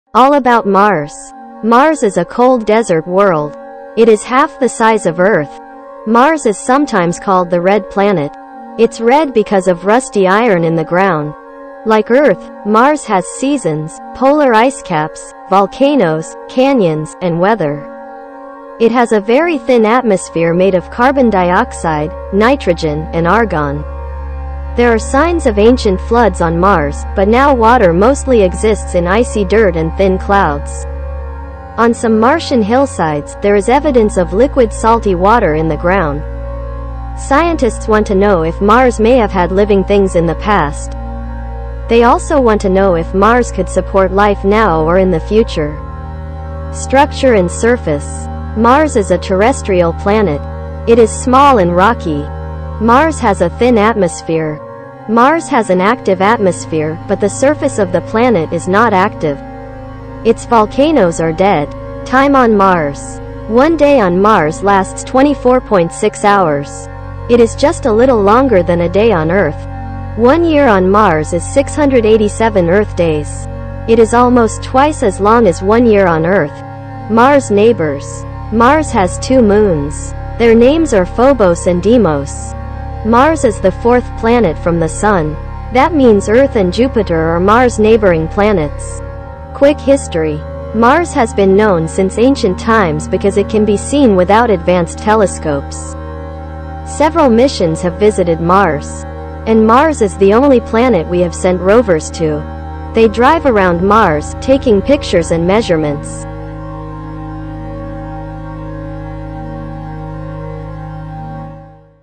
All About Mars | Solar System | 3D 2023 | Tell me all about Mars
Data taken from Google:

All About Mars

Mars is a cold desert world. It is half the size of Earth.
Mars is sometimes called the Red Planet. It's red because of rusty iron in the ground.

Like Earth, Mars has seasons, polar ice caps, volcanoes, canyons, and weather.
It has a very thin atmosphere made of carbon dioxide, nitrogen, and argon.

There are signs of ancient floods on Mars, but now water mostly exists in icy dirt and thin clouds.
 On some Martian hillsides, there is evidence of liquid salty water in the ground.

Scientists want to know if Mars may have had living things in the past.
They also want to know if Mars could support life now or in the future.

Structure and Surface

Mars is a terrestrial planet. It is small and rocky.

Mars has a thin atmosphere.

Mars has an active atmosphere, but the surface of the planet is not active. Its volcanoes are dead.

Time on Mars

One day on Mars lasts 24.6 hours. It is just a little longer than a day on Earth.

One year on Mars is 687 Earth days. It is almost twice as long as one year on Earth.

Mars’ Neighbors

Mars has two moons. Their names are Phobos and Deimos.

Mars is the fourth planet from the Sun. That means Earth and Jupiter are Mars’ neighboring planets.

Quick History

Mars has been known since ancient times because it can be seen without advanced telescopes.

Several missions have visited Mars. And Mars is the only planet we have sent rovers to.
They drive around Mars, taking pictures and measurements.